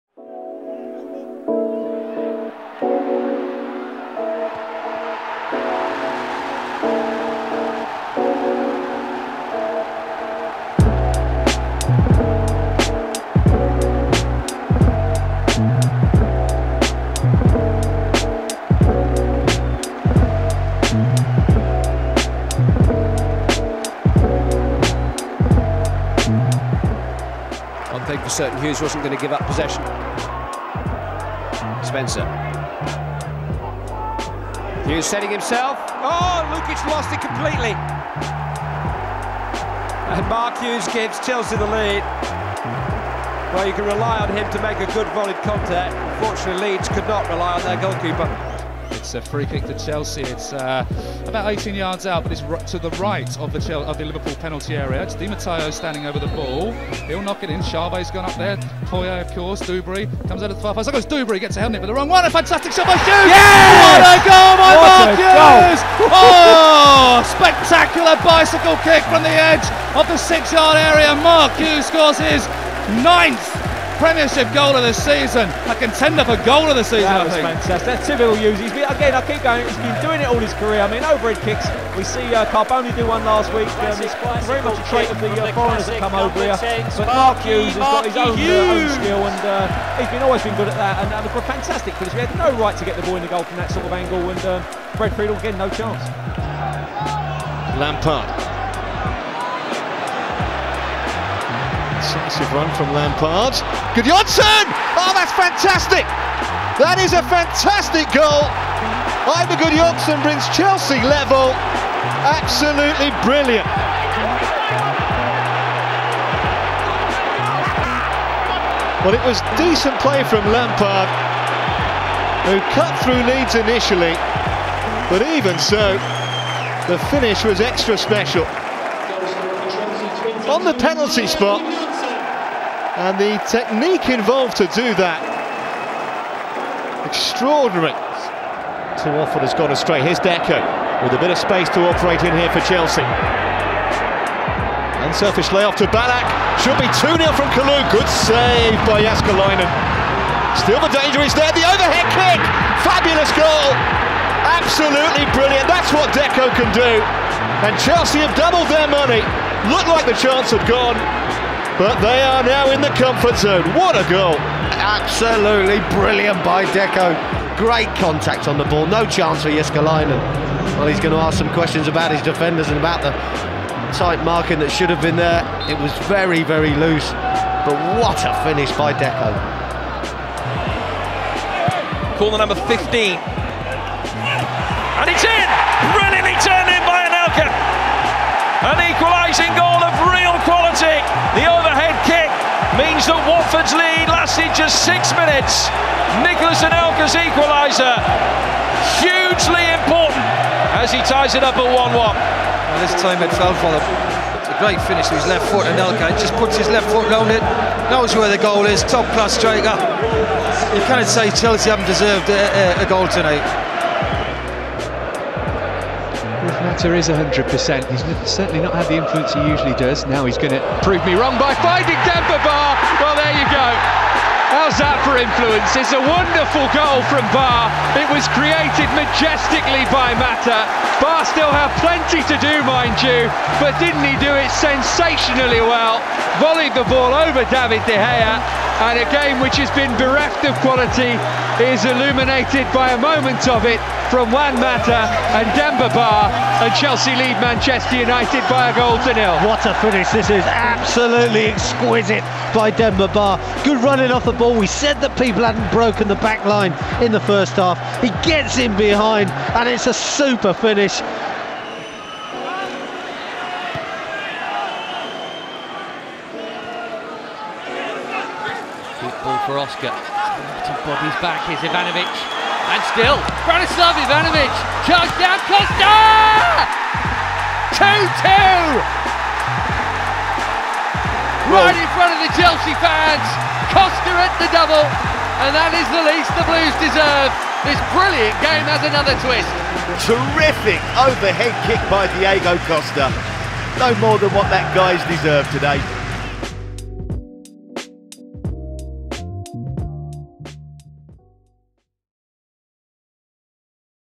Chelsea's Most Memorable Acrobatic Goals | Costa, Anelka, Deco & More
Take a look back at a selection of Chelsea's best acrobatic goals from oldest to the most recent. Let us know which one is your favourite.